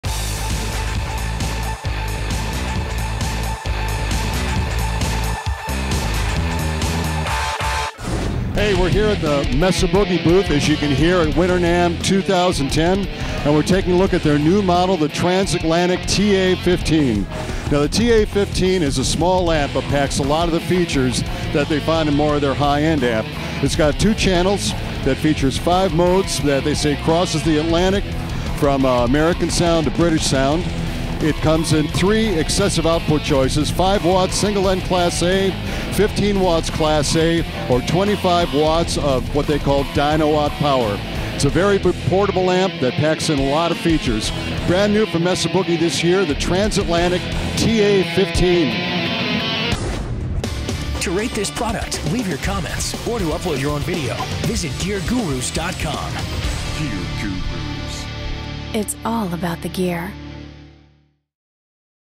Mesa Engineering TA-15 Amp - New at NAMM Winter 2010
Check out this awesome new mini stack by Mesa Engineering. The TA-15 is at NAMM Winter 2010.